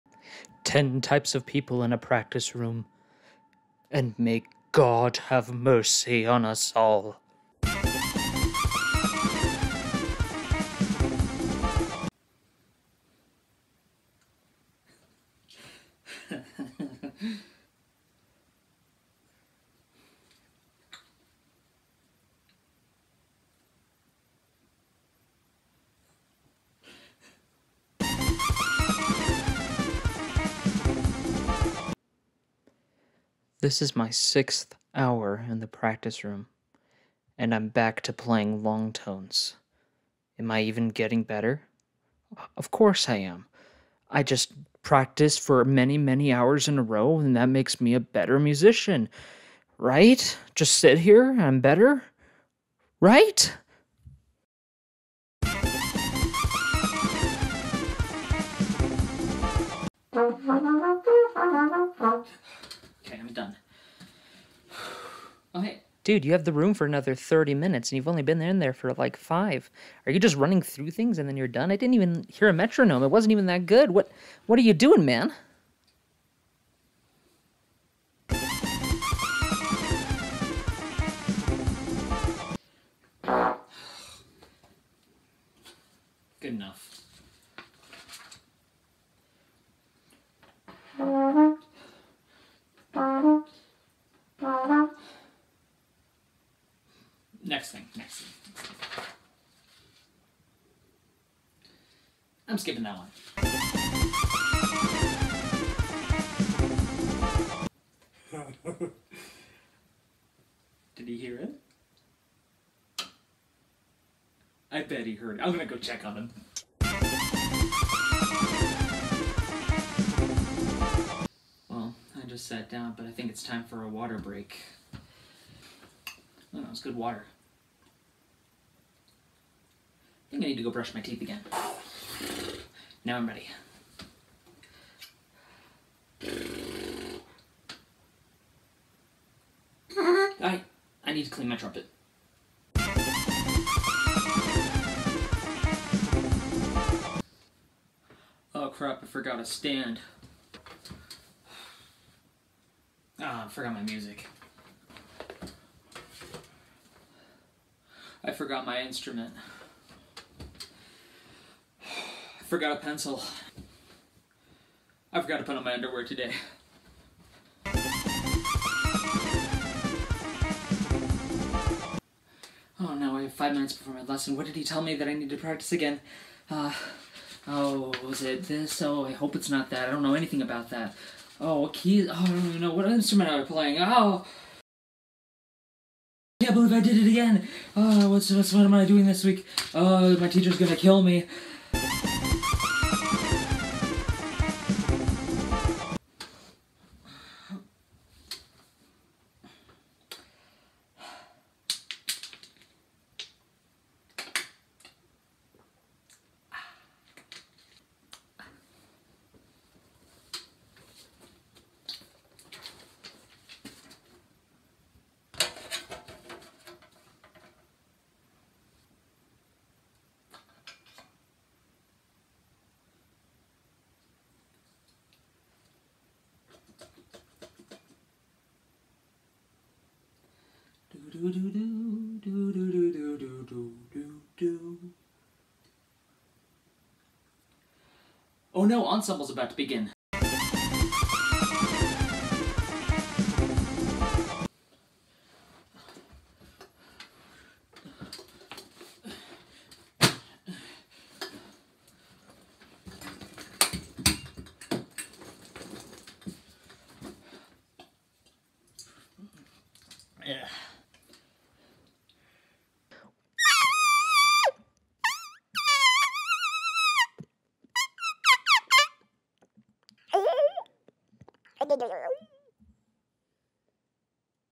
10 types of people in a practice room
If you have never been at least one of these...you might be lying...or you are a being beyond comprehension.